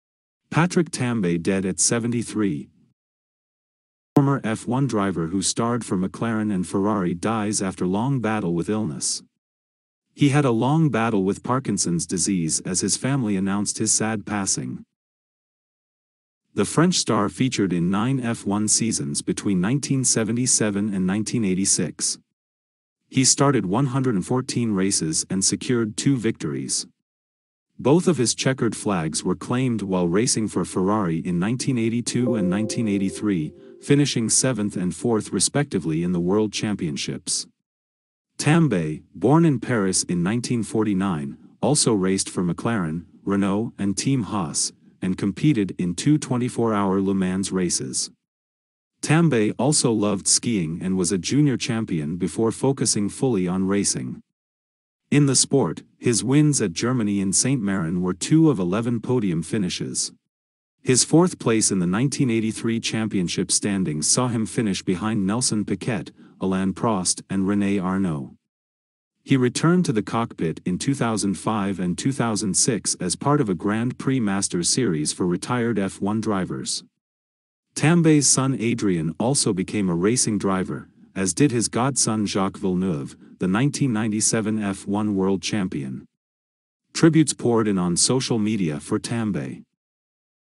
Patrick Tambay dead at 73
He had a long battle with Parkinson's disease as his family announced his sad passing.
The French star featured in nine F1 seasons between 1977 and 1986.
He started 114 races and secured two victories.
Both of his chequered flags were claimed while racing for Ferrari in 1982 and 1983, finishing seventh and fourth respectively in the world championships.
Tambay, born in Paris in 1949, also raced for McLaren, Renault and Team Haas - and competed in two 24-hour Le Mans races. Tambay also loved skiing and was a junior champion before focusing fully on racing.
In the sport, his wins at Germany and Saint-Marin were two of 11 podium finishes.
His fourth place in the 1983 championship standings saw him finish behind Nelson Piquet, Alain Prost and Rene Arnoux.IMPORTANT!